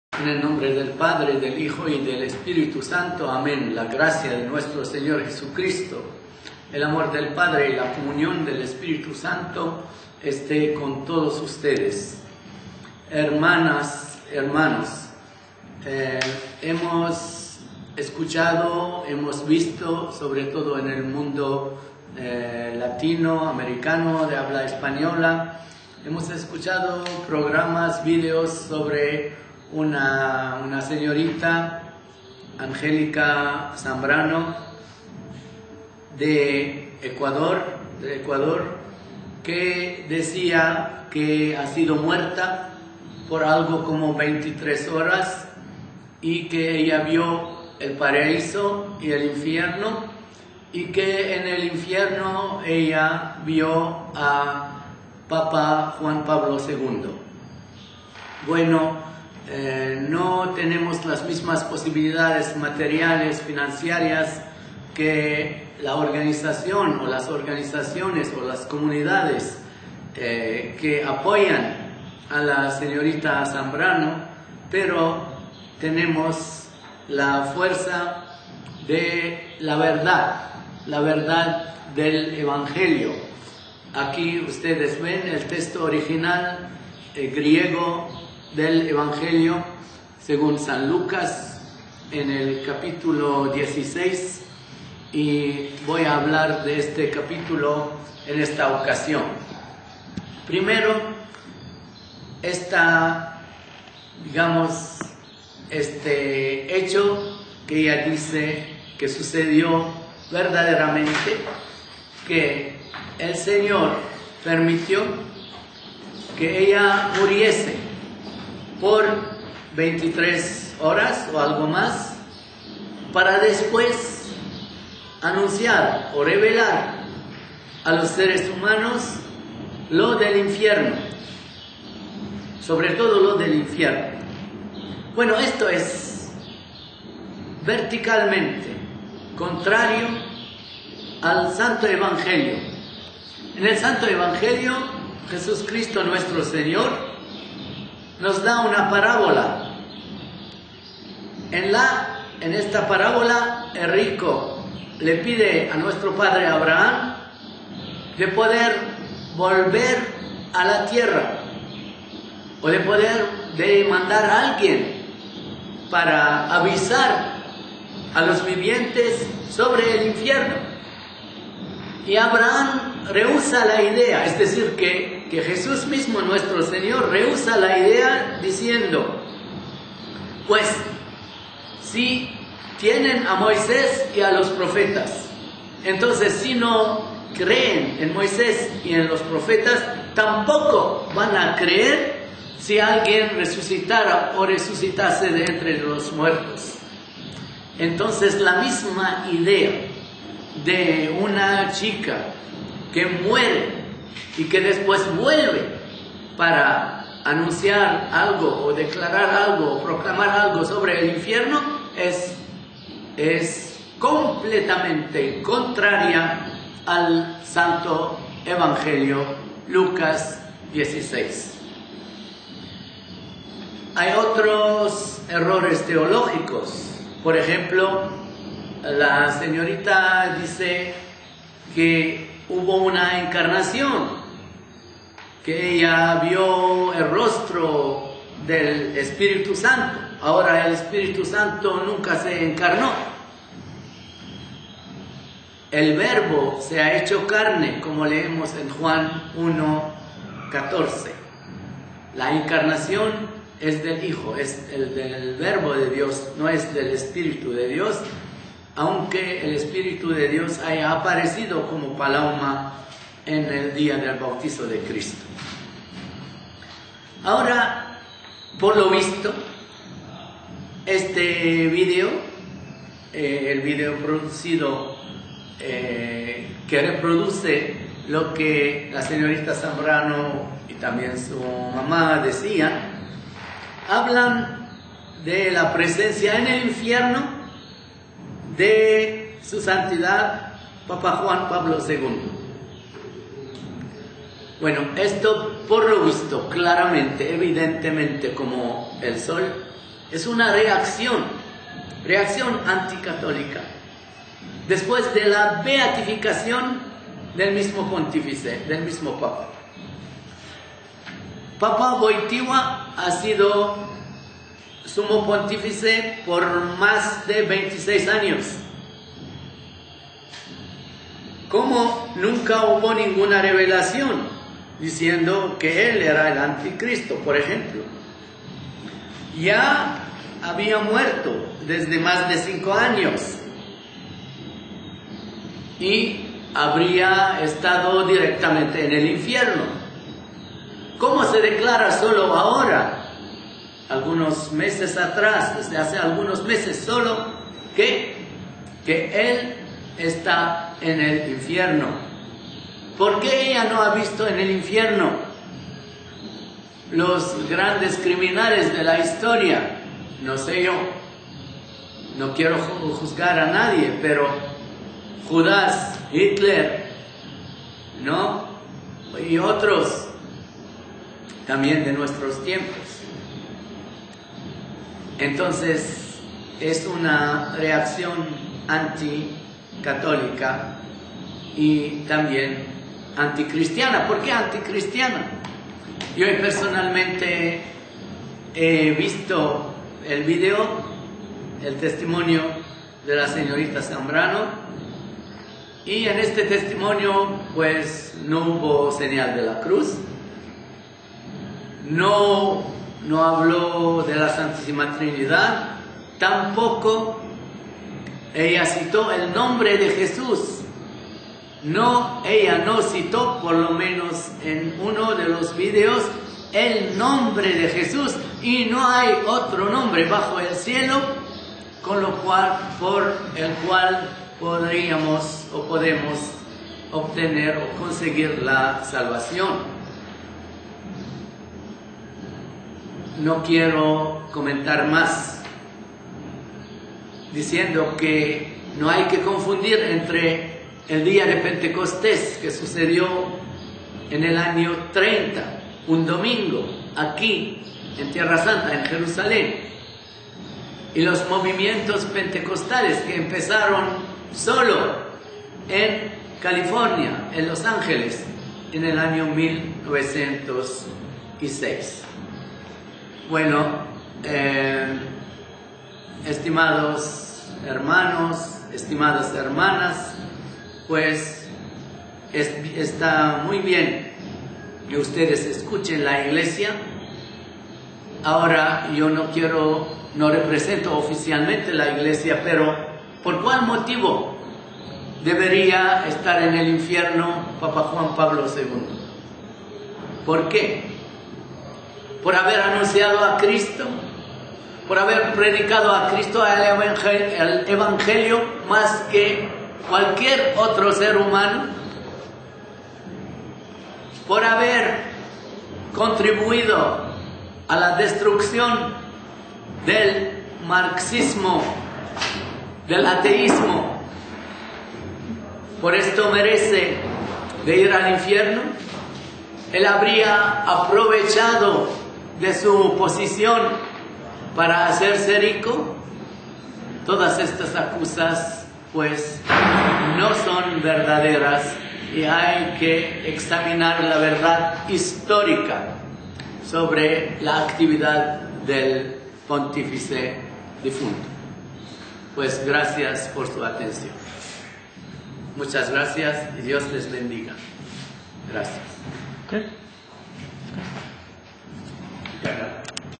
Re: (Spanish) "23 Hours Dead" - Testimony of Angelica Zambrano
A response to the video that talks about a girl trip to hell and heave in different languages.